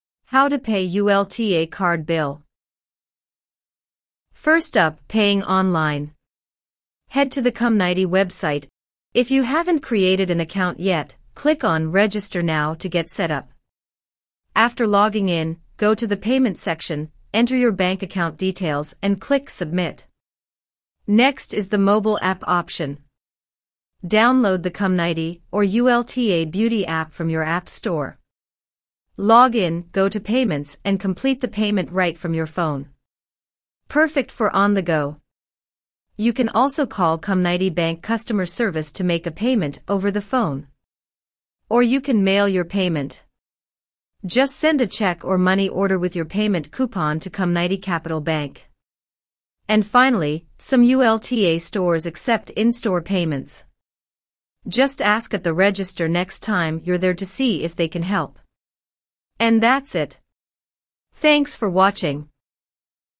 How to pay ULTA card l DOUBLE Z
Need to make a payment on your Ulta Credit Card? This video walks you through all the ways to pay, whether online, by phone, or in-store! Learn how to manage your card on the Ulta app or website, schedule payments, and avoid late fees. We'll cover:

Steps for online and mobile app payments.
How to set up automatic payments for convenience.
Options for paying by phone or mail.
Ideal for Ulta shoppers in the USA, UK, Canada, and Europe looking for easy and efficient payment methods. Get on top of your Ulta card payments and keep enjoying beauty rewards!